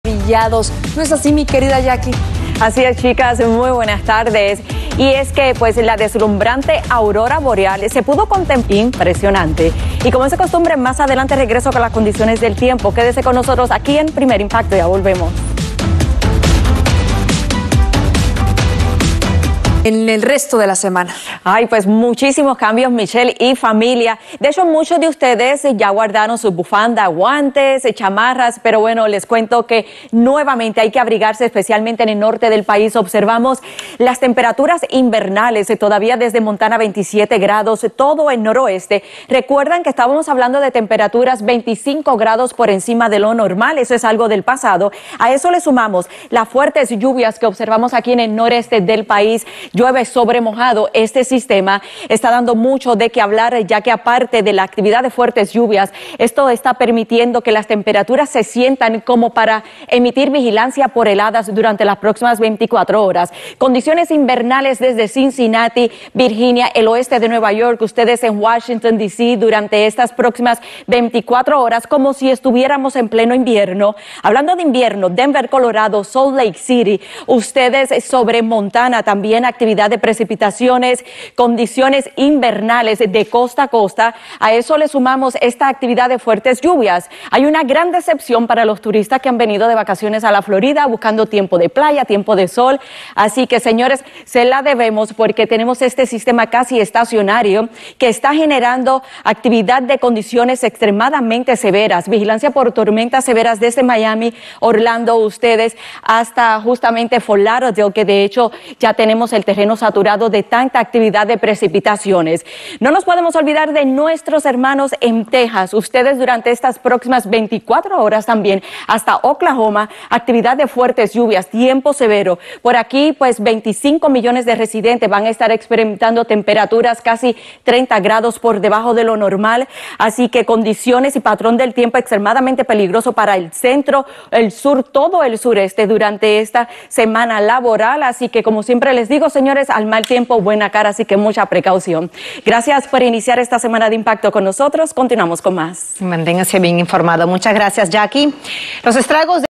Jackie Guerrido 04 24 2023
Jackie Guerrido delivers the forecast, wearing a purple dress!